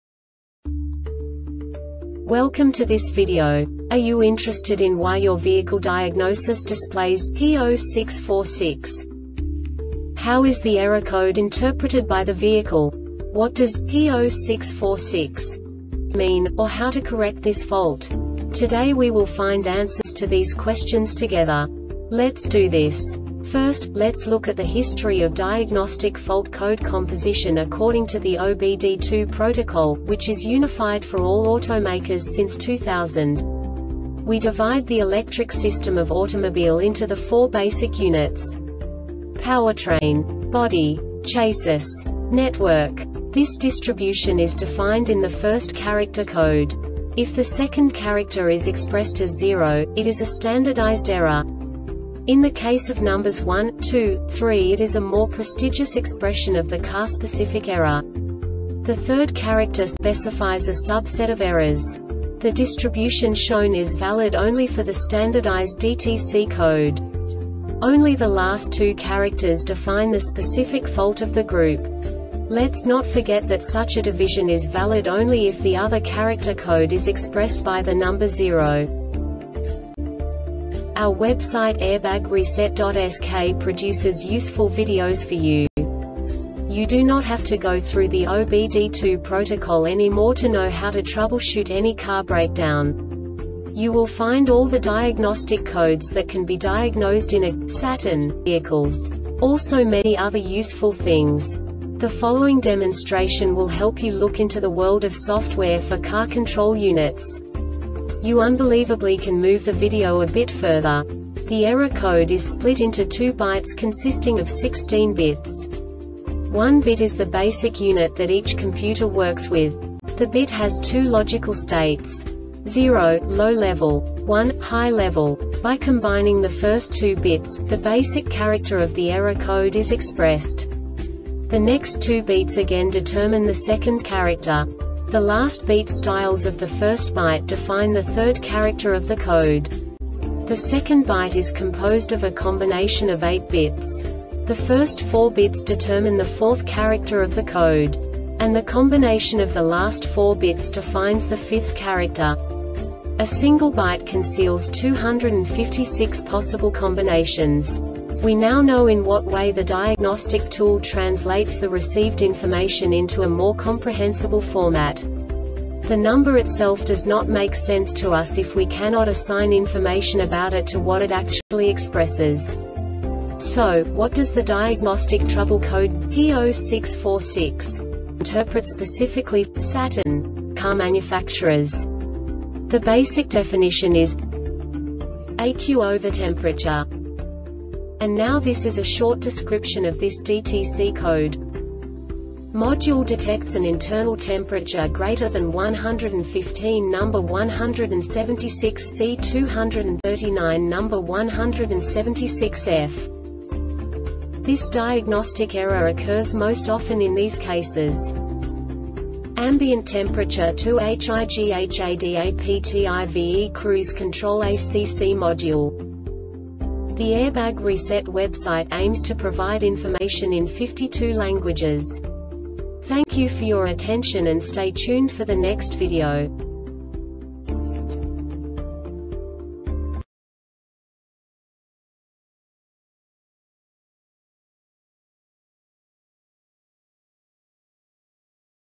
DTC Saturn P0646 Short Explanation
Contents:
0:21 Basic DTC analysis according to OBD2 protocol standard.
1:48 Insight into programming
2:58 A diagnostically understandable description of the Diagnostic Trouble Code
3:05 Basic error definition

Make:
Saturn
Code:
P0646
Definition:
ECU Overtemperature
Description:
Module detects an internal temperature greater than 115&#176;C (239&#176;F).
Cause:
AMBIENT TEMPERATURE TOO HIGHADAPTIVE CRUISE CONTROL (ACC) MODULE